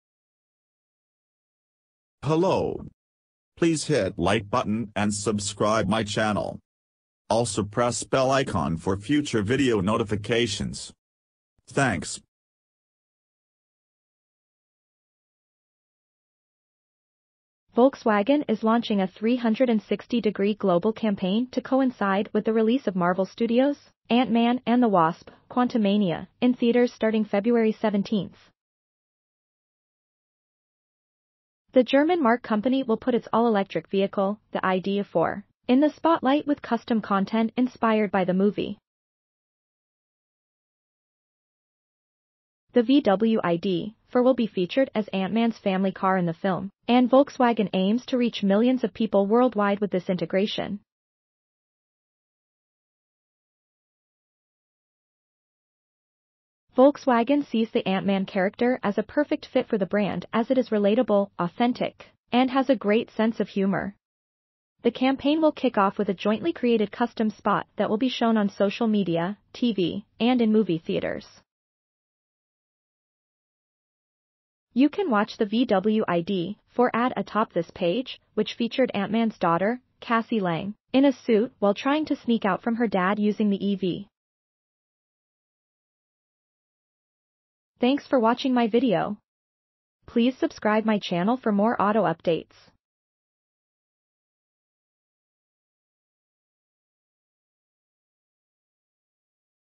VW ID.4 Is Ant-Man's Shrinkable Family Vehicle In New Campaign
Volkswagen is launching a 360-degree global campaign to coincide with the release of Marvel Studios' "Ant-Man and The Wasp: Quantumania" in theaters starting February 17. The German marque company will put its all-electric vehicle, the ID.4, in the spotlight with custom content inspired by the movie. The VW ID.4 will be featured as Ant-Man's family car in the film, and Volkswagen aims to reach millions of people worldwide with this integration. Volkswagen sees the Ant-Man character as a perfect fit for the brand as it is relatable, authentic, and has a great sense of humor. The campaign will kick off...